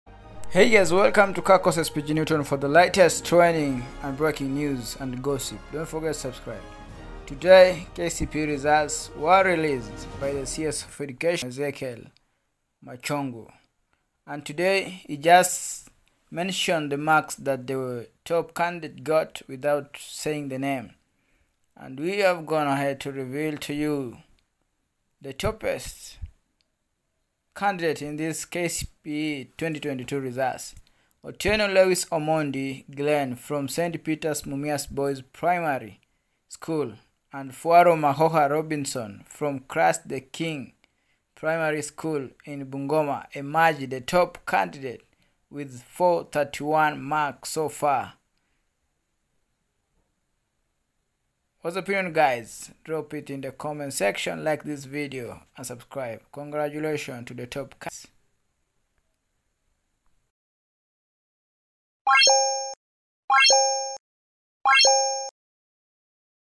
Top KCPE candidate 2022 "Revealed "
KCPE 2022: Otieno Lewis Omondi Glen from St Peters Mumias Boys Primary School and Fwaro Makokha Robinson from Christ the King Primary School in Bungoma emerge top candidates with 431 marks.

Congratulations to OTIENO Lewis Omondi from St Peter's Mumias Boys Primary school for being the top student in Kenya,KCPE 2022.Congratulations to all Learners who sat for their Exams.There are no shortcuts to any place worth going.MATHEMATICS has topped the list na tunasema TIBIM